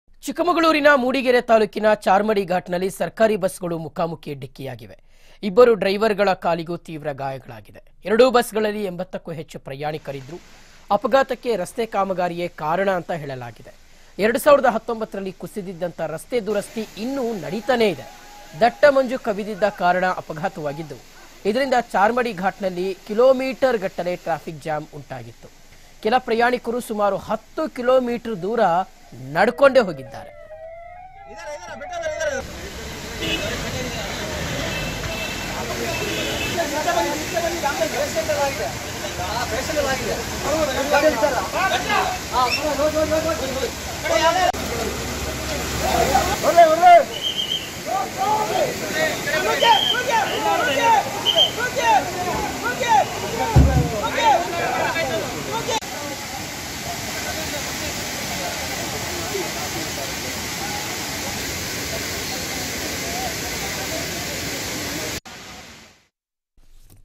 ಚಿಕ್ಕಮಗಳೂರಿನ ಮೂಡಿಗೆರೆ ತಾಲೂಕಿನ ಚಾರ್ಮಾಡಿ ಘಾಟ್​ನಲ್ಲಿ ಸರ್ಕಾರಿ ಬಸ್ಸುಗಳು ಮುಖಾಮುಖಿ ಡಿಕ್ಕಿಯಾಗಿವೆ!
Btv News Kannada : ಚಿಕ್ಕಮಗಳೂರಿನ ಮೂಡಿಗೆರೆ ತಾಲೂಕಿನ ಚಾರ್ಮಾಡಿ ಘಾಟ್​ನಲ್ಲಿ ಸರ್ಕಾರಿ ಬಸ್ಸುಗಳು ಮುಖಾಮುಖಿ ಡಿಕ್ಕಿಯಾಗಿವೆ!